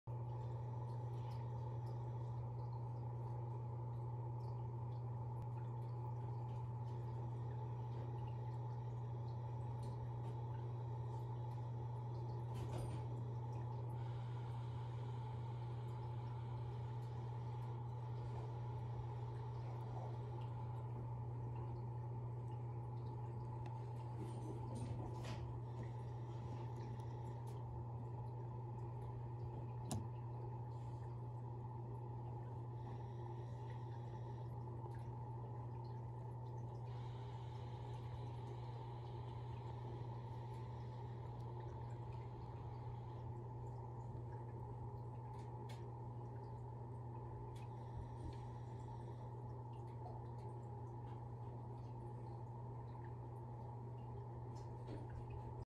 Mystery snail On The Move
I am entranced by how my Mystery Snail Ivory moves along the bottom of the fish tank.
 Her one giant foot that propels her is as elegant as an evening gown caught up in a gentle wind. She slides along with such ease, contouring her foot to match the uneven surface in one continuous movement.
At times she seems to float but in reality, her foot never leaves contact with the surface she's on.
Then Ivory defies gravity, settling on the pink leaf, as if weightless. fullmoonfiberart.com